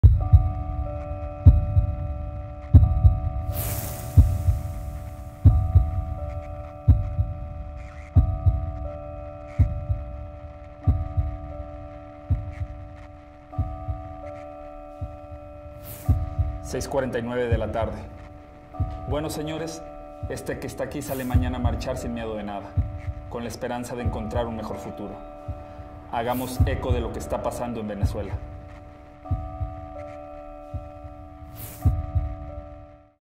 Manolo Caro. Eco del joven asesinado Bassil Da Costa. 12-02-2014. Venezuela
El actor hace eco desde México del estatus de Facebook del joven Bassil Da Costa, asesinado en Caracas en una protesta el 12 de Febrero. Hagamos eco de lo que está pasando en Venezuela.